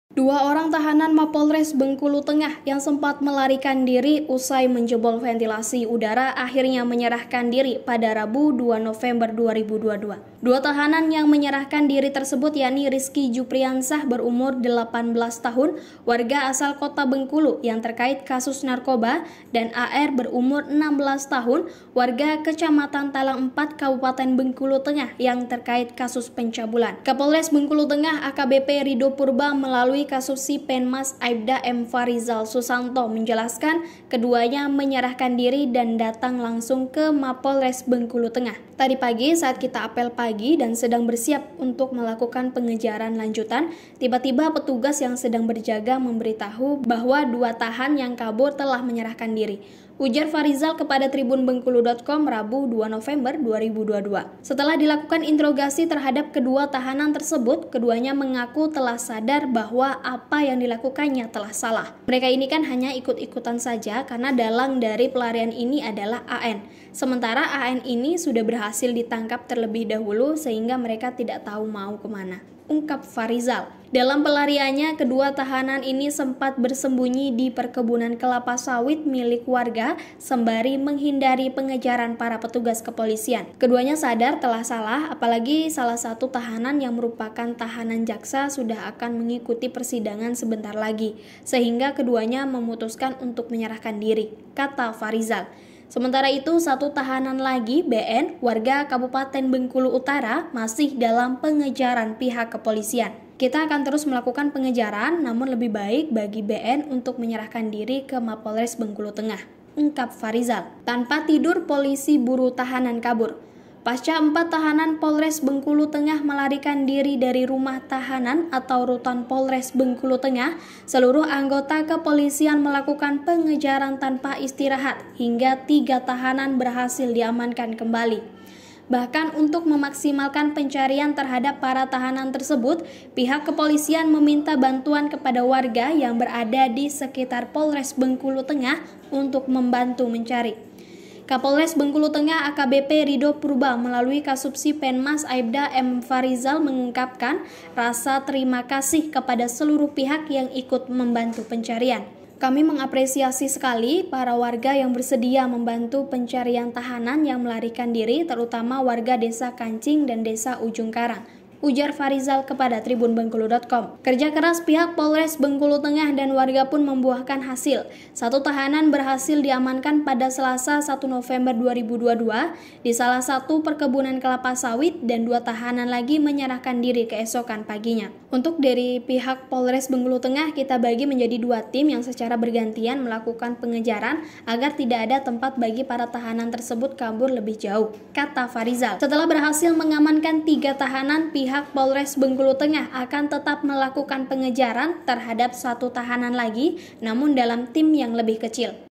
Bingung Mau Kabur Kemana, 2 Pelaku dari 4 Tahanan Polres Bengkulu Tengah yang Kabur Serahkan Diri
TRIBUNBENGKULU.COM, BENGKULU TENGAH - Dua orang tahanan Mapolres Bengkulu Tengah yang sempat melarikan diri usai menjebol ventilasi udara akhirnya menyerahkan diri, pada Rabu (2/11/2022).

Dua tahanan yang menyerahkan diri tersebut yakni Riski Jupriansah (18) warga asal Kota Bengkulu yang terkait kasus narkoba dan AR (16) warga Kecamatan Talang Empat Kabupaten Bengkulu Tengah yang terkait kasus pencabulan.